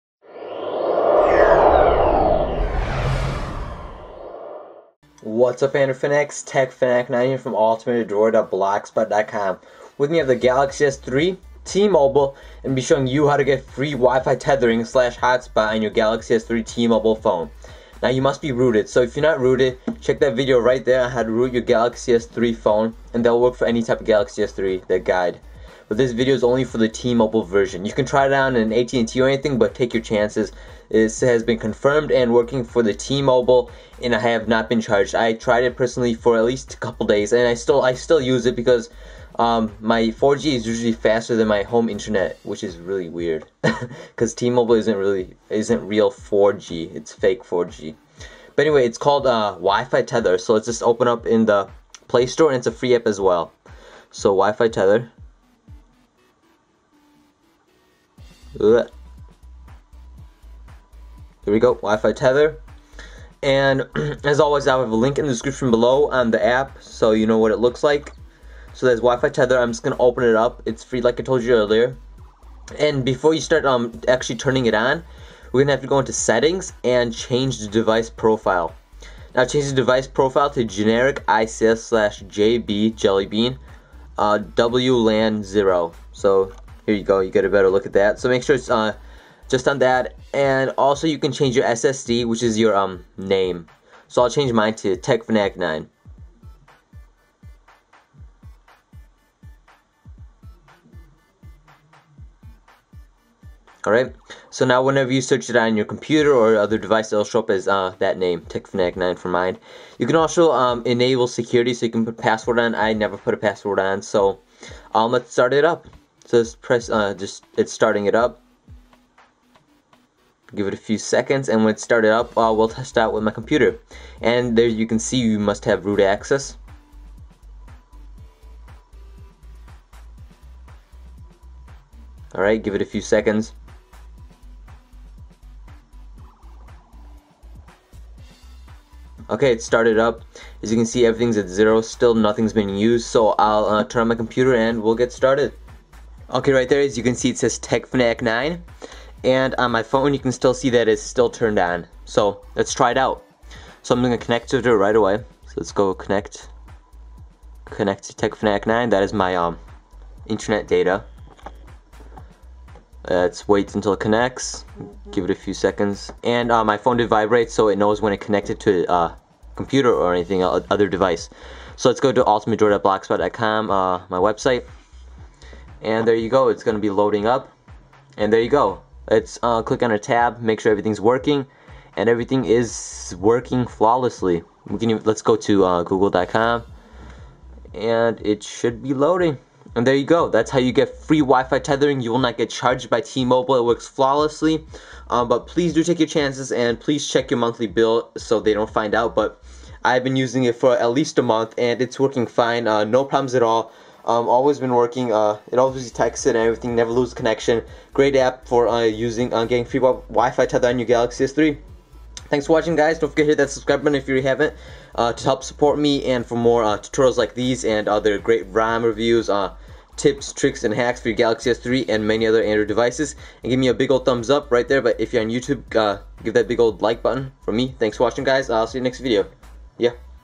How to Get Free WiFI Tether / Hotspot on T-Mobile Samsung Galaxy S3! [ICS]
Here's how to get free Wifi tether on T-Mobile SGH-T999 Samsung Galaxy S3.